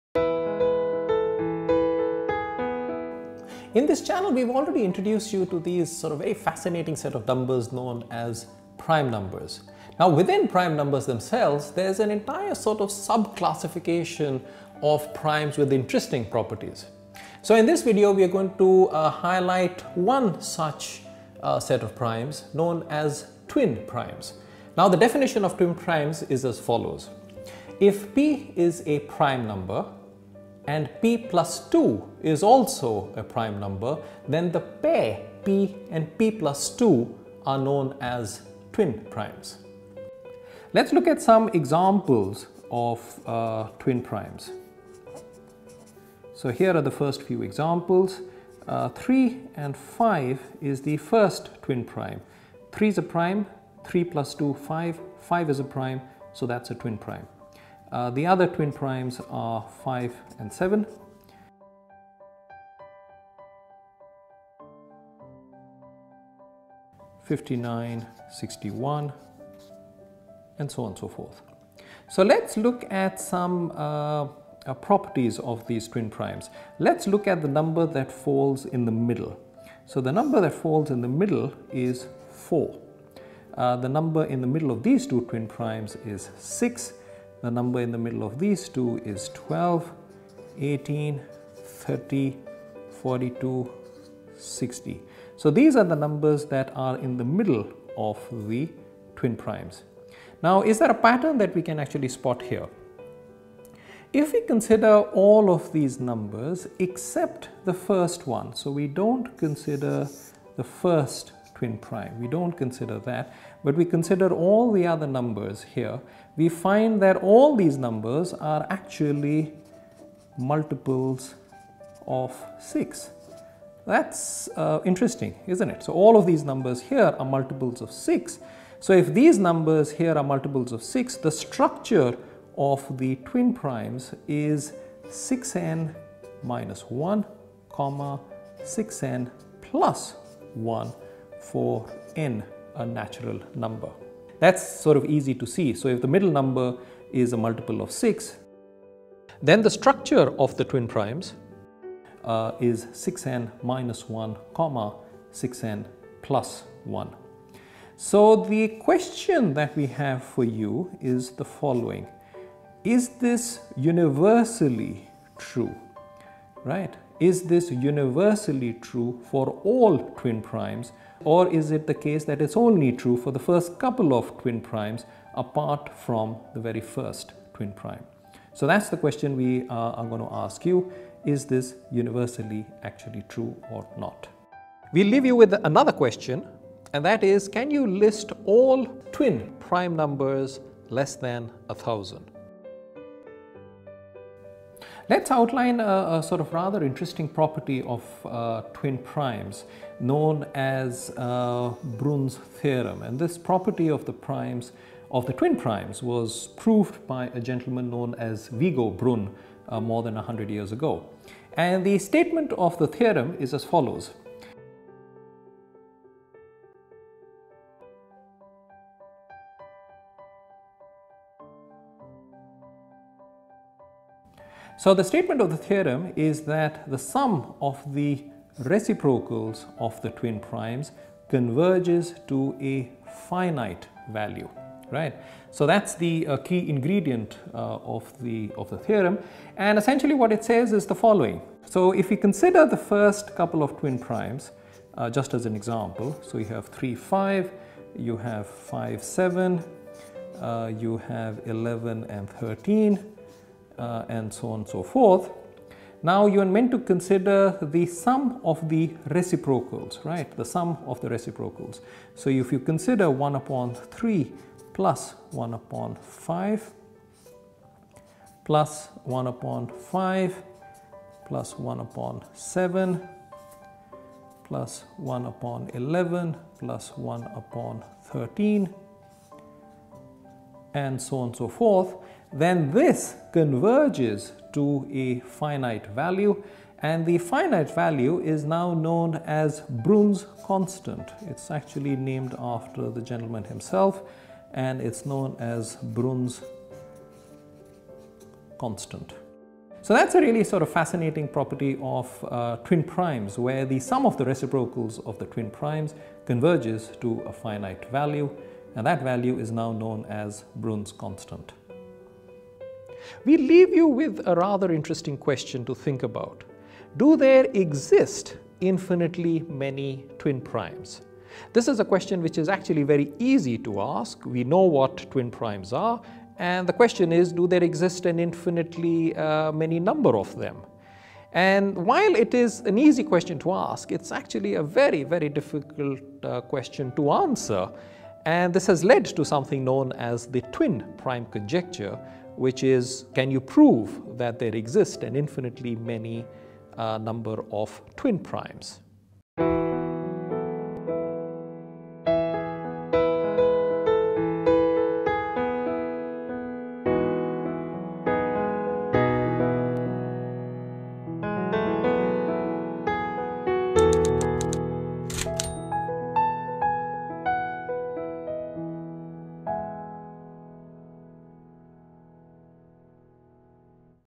Twin Prime numbers
What are Twin Prime numbers? What are the different types of prime numbers?